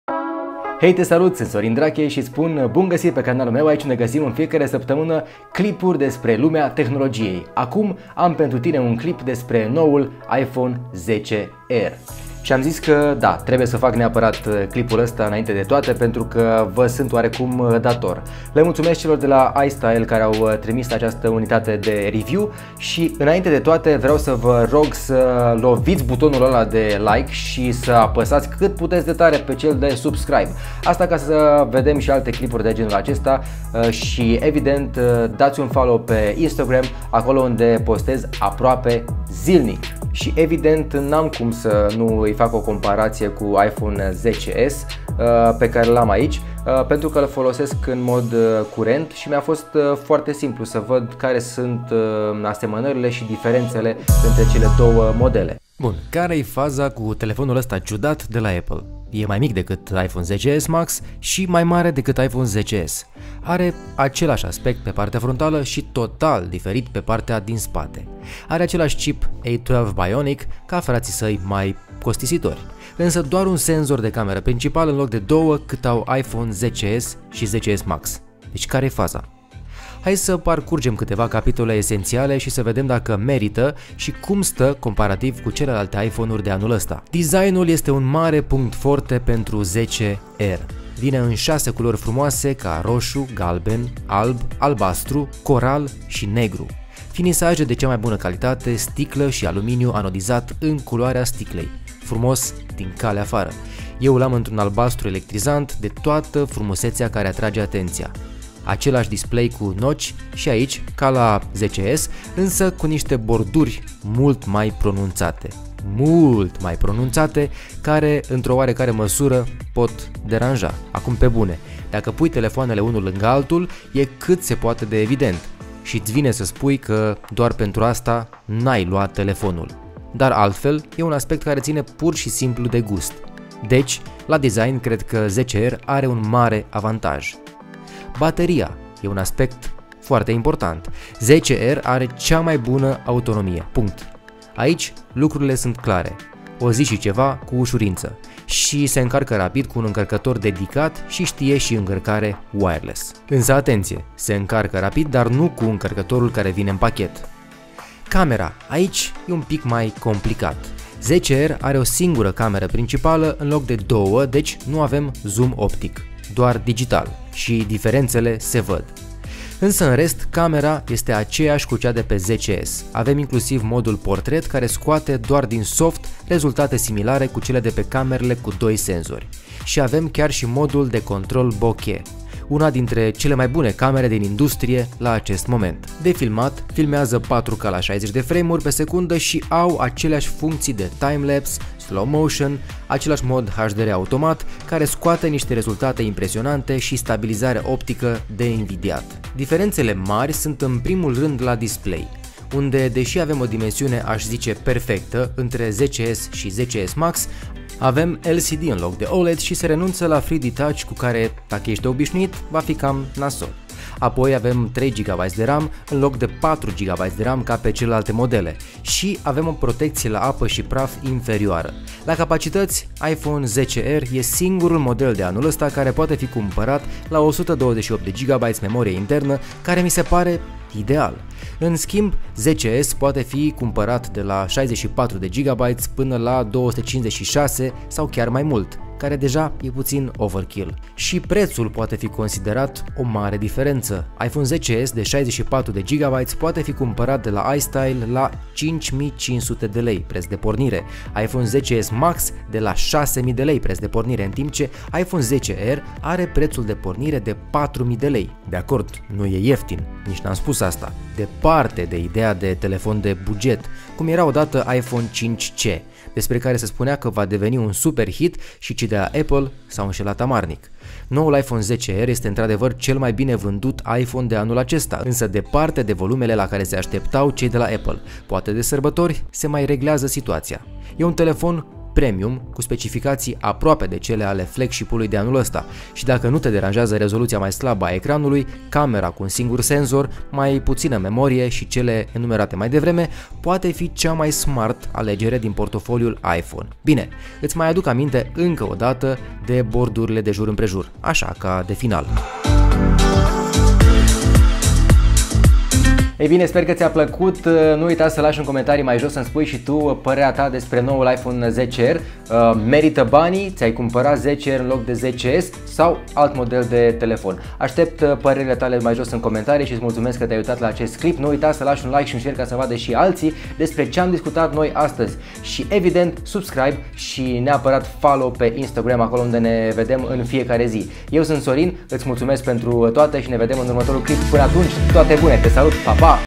Adevarul despre iPhone XR
Cel mai bine vandut model de iPhone de anul acesta. Dar, oare este destul pentru a fi noul tau telefon?

Review pentru iPhoneXR prezentat de cei de la iStyle.ro.

Nu uita sa te inscrii in lista mea de prieteni pentru continut exclusiv, giveaways, behind the scenes si altele: